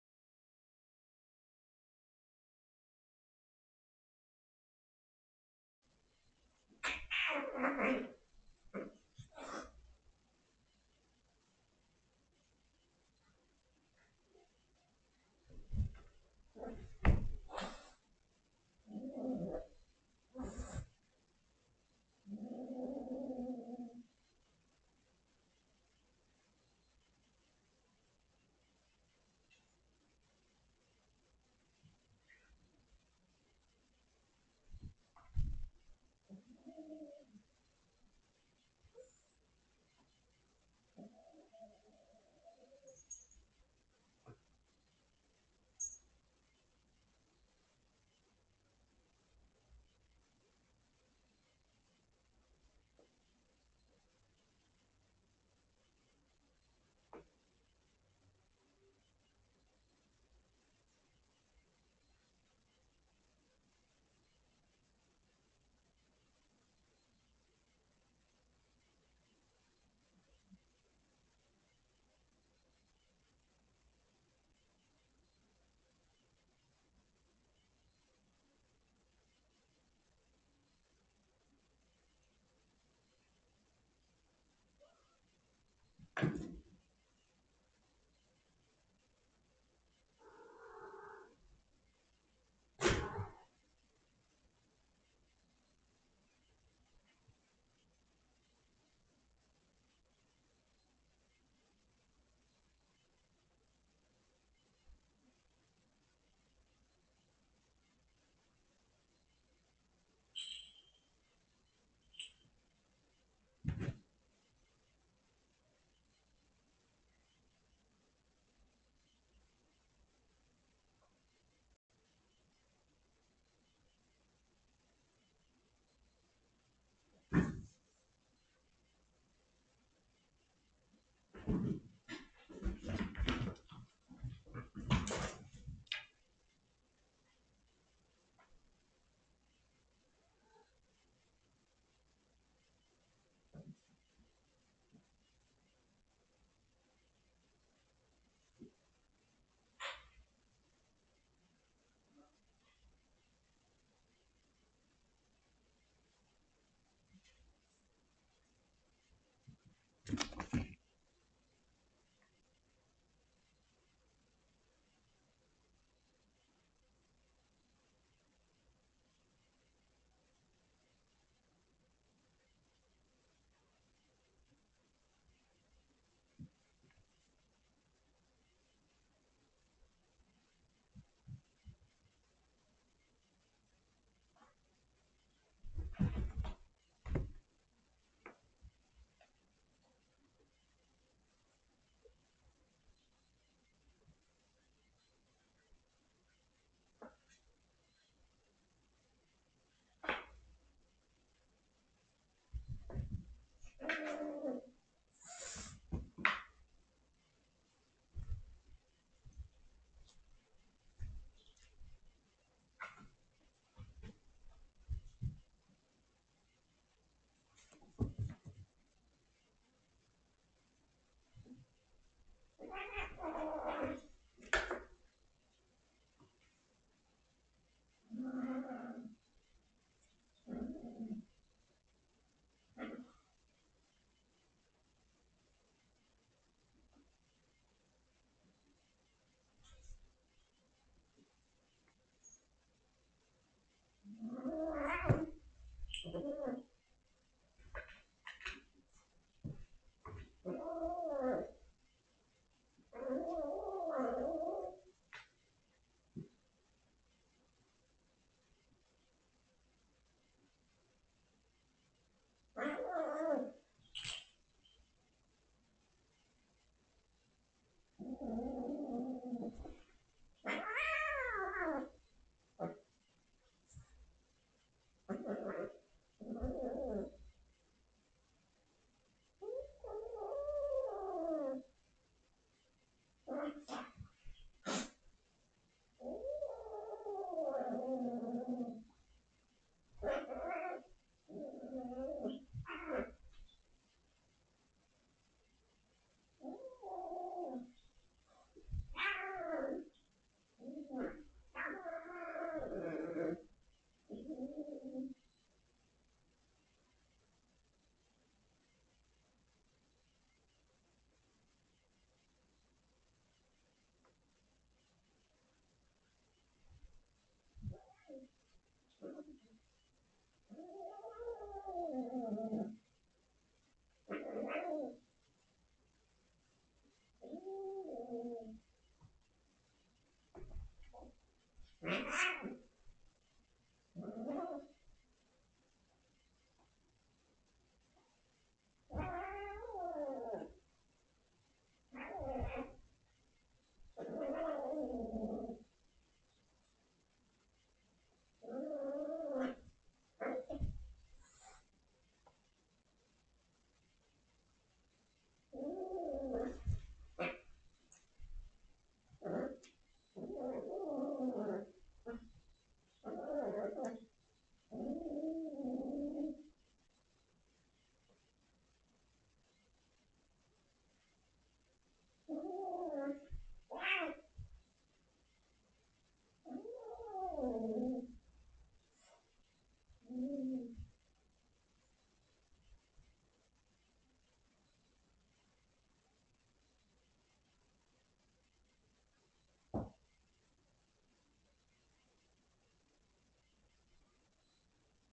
2023-09-02 Micro
CHAPTERS:
00:00 - [Intro & Petty Cash meet crunchy treats]
02:01 - [Till is happy to clean up any treats]